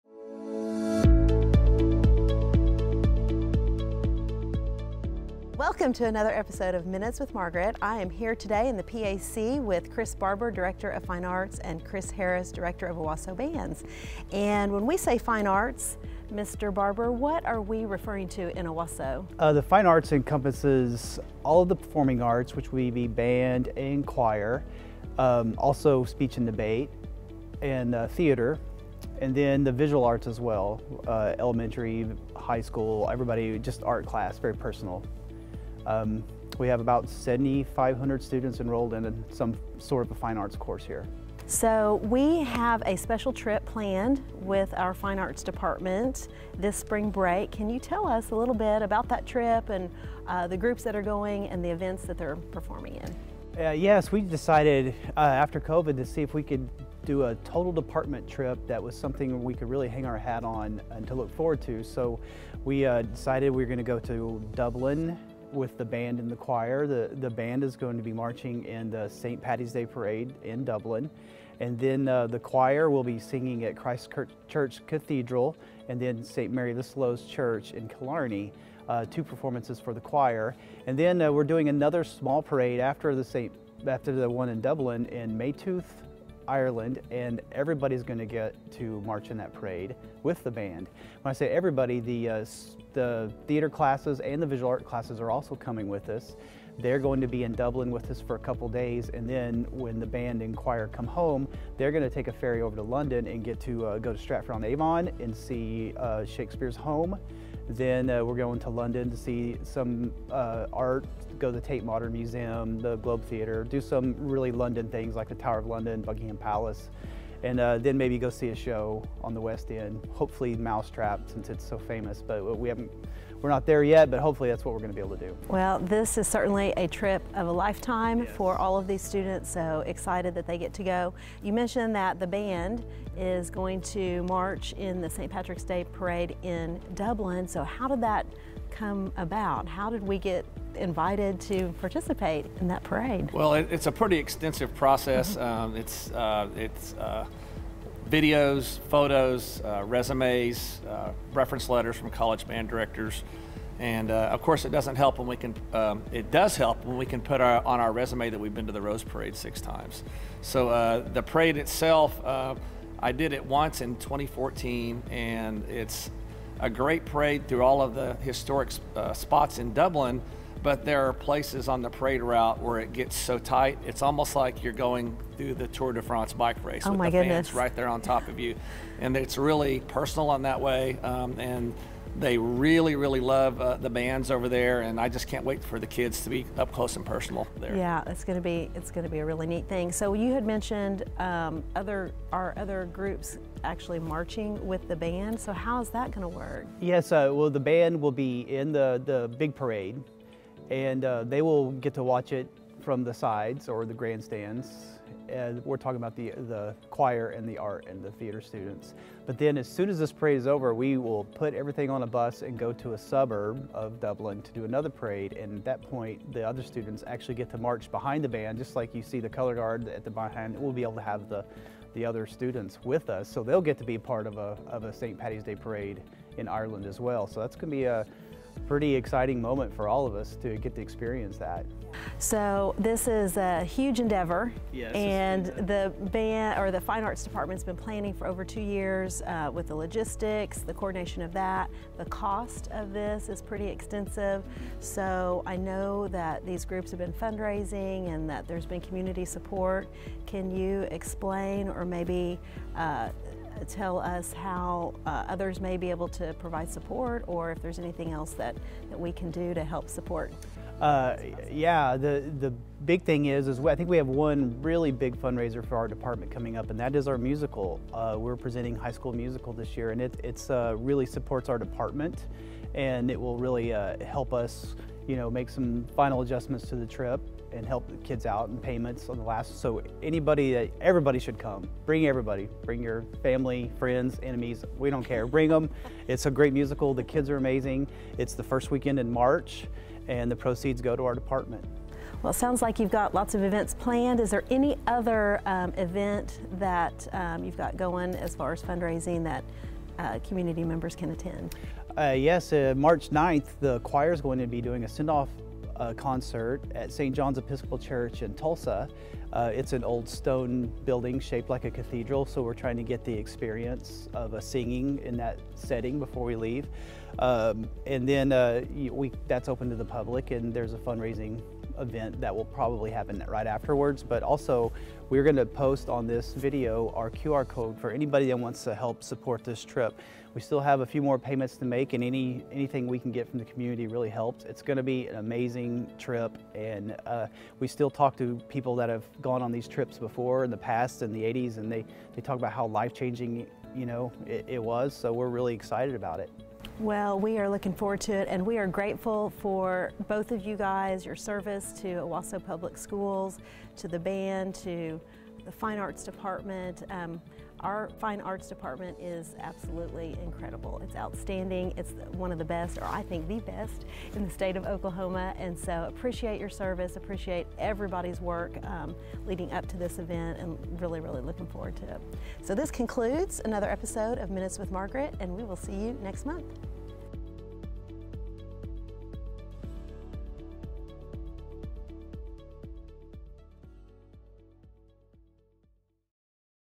Minutes with Margaret - Episode 6 - Fine Arts
It’s time for another episode of Minutes with Margaret! This month’s episode includes Director of Fine Arts Chris Barber and Director of Bands Chris Harris to talk about Fine Arts in the district and the department’s upcoming trip to Ireland & London over Spring Break. Check it out!